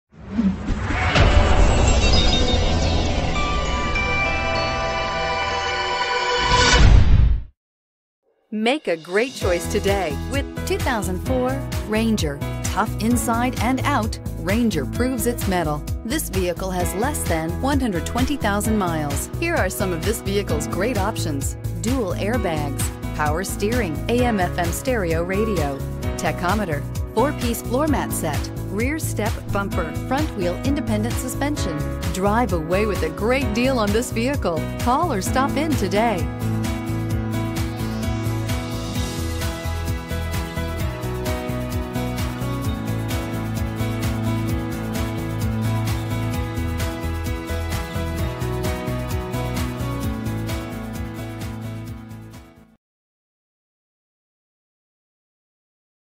2004 Ford Ranger Lynwood WA 30844
2004 Ford Ranger XL

Carson Cars
13806 Highway 99

Pick up this Oxford White Clearcoat 2004 Ford Ranger, available today at Carson Cars. This could be the one you've been searching for.